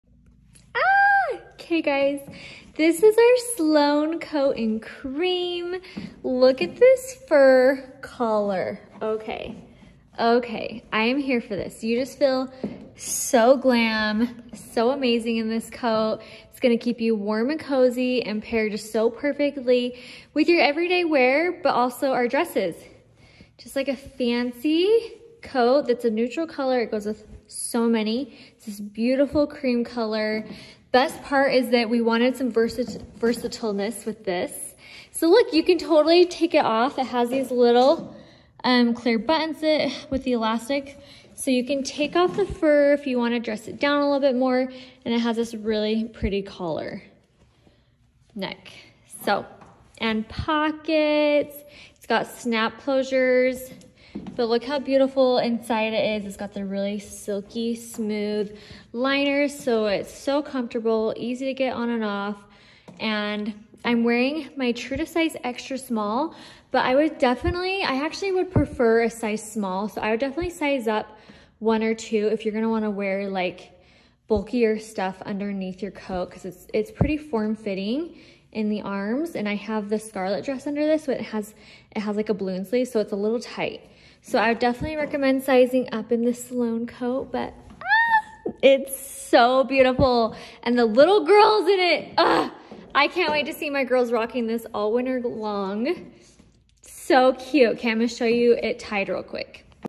Sloane Coat in Cream Try On in Size XS
The coat-of-the-year is finally here! With cream coloring, and the perfect mid calf length, this coat is sure to be your number one layering pick this season. Made from the comfiest and classiest fabric, with a waist belt and detachable faux-fur collar, this trench has two looks wrapped into one! Best news is we have it in a matching MINI also! Available in size XXS, XS, S, M, L, XL, XXL, 1X, 2X, & 3X + matching mini sizes!